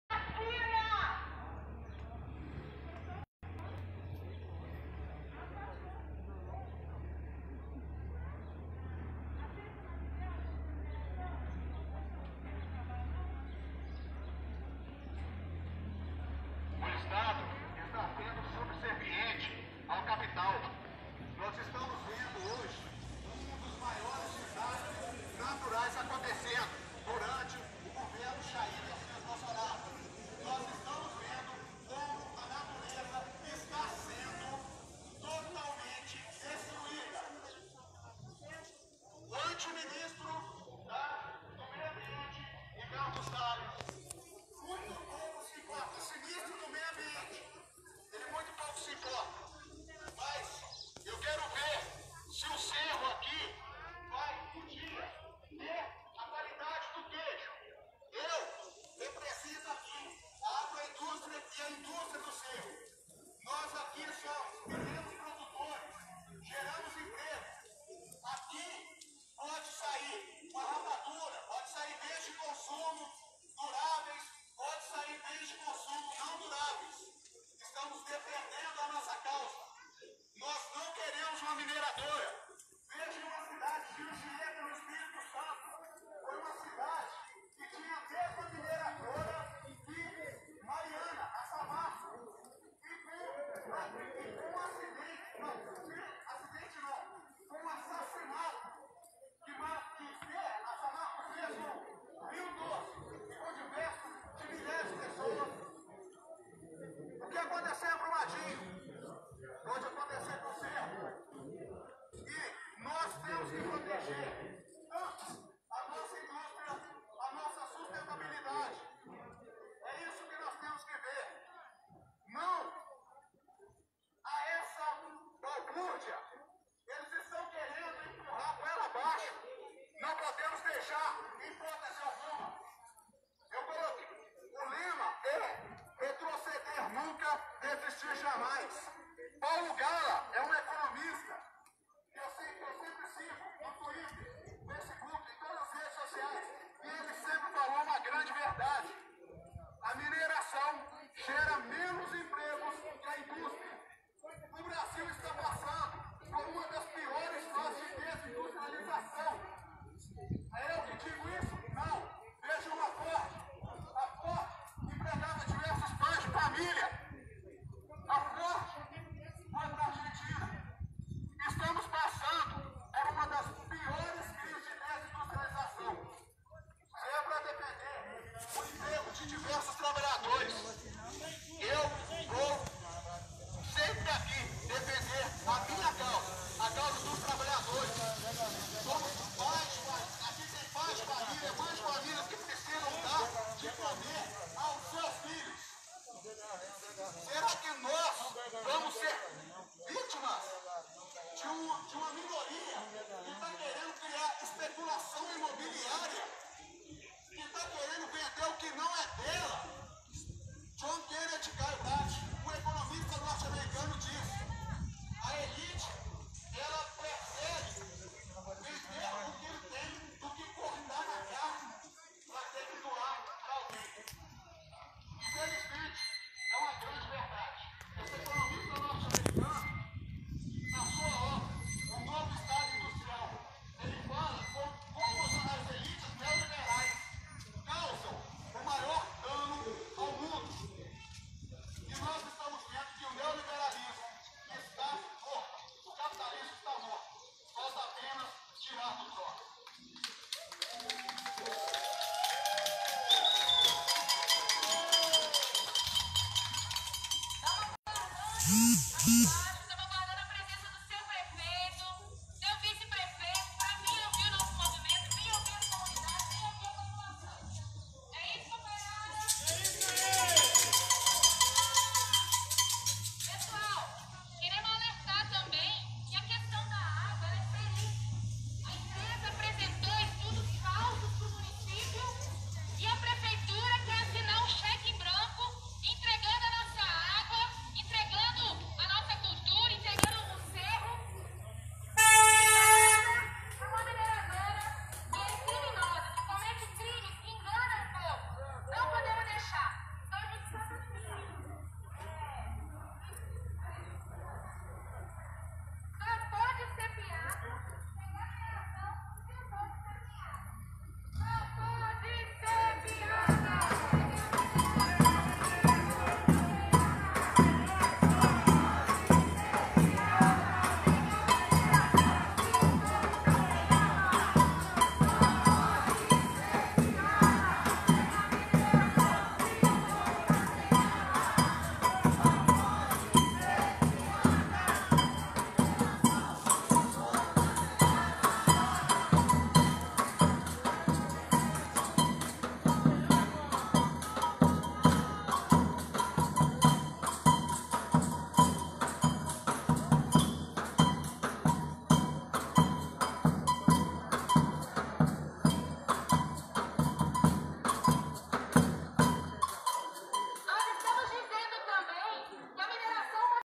Protesto contra a mineração no Serro dia 01/02/2021
Participei do protesto contra a instalação da Herculano Mineração no Serro , que dada a carta de conformidade por motivos fraudulentos e um Codema que agiu de maneira favorável a instalação da mineradora na cidade . Seguiremos contestando a decisão .
Vídeo de Sidney Dutra .